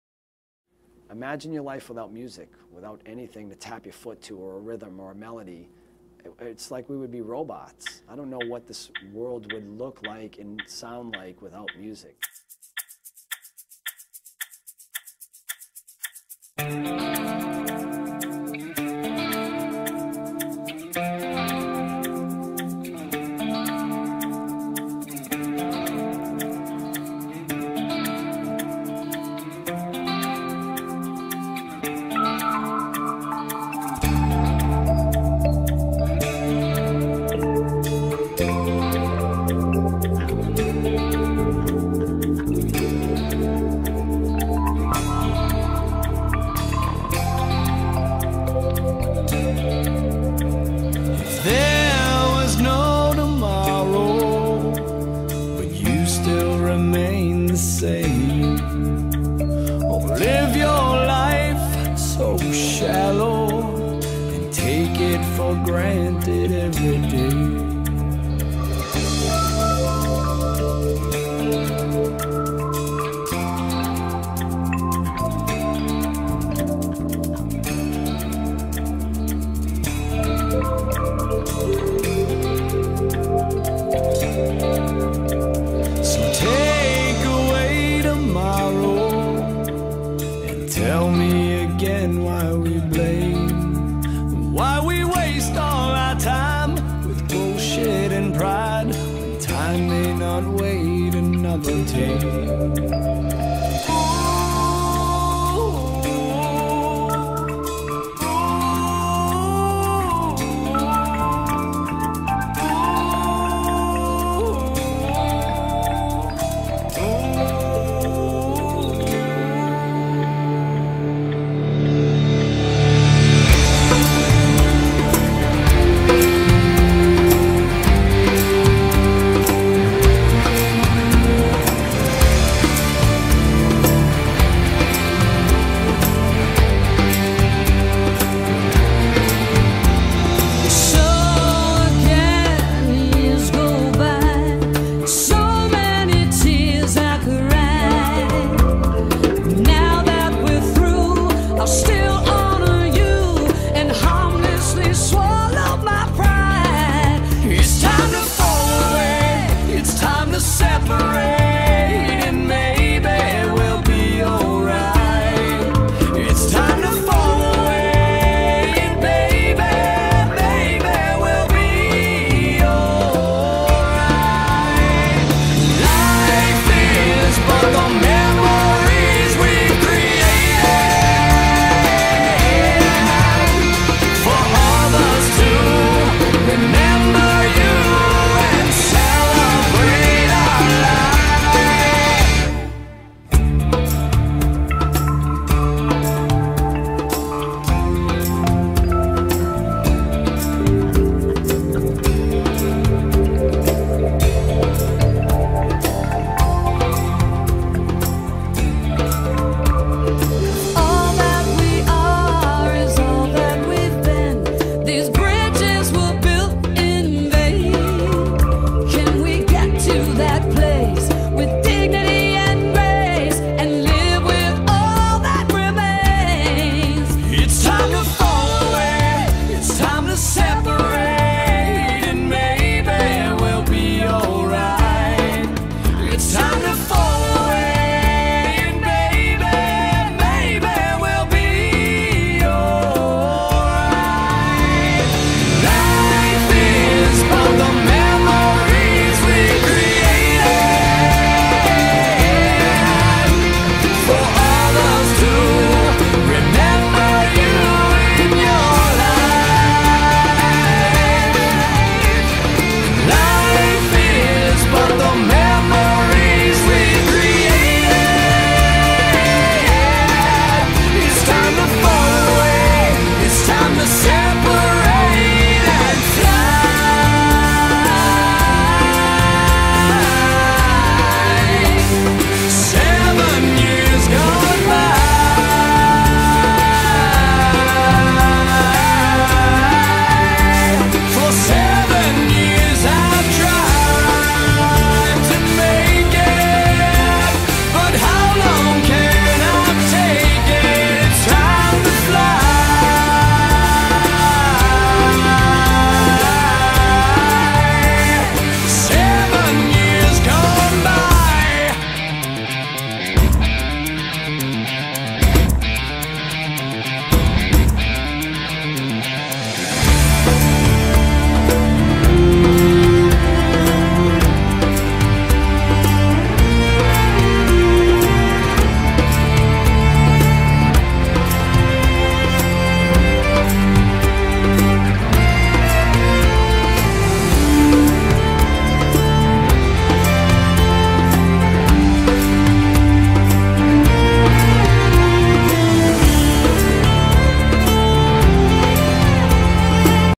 My Paintings - Dawn Arnold
This is a video of my paintings. to of course Sully Erna, I just love this song!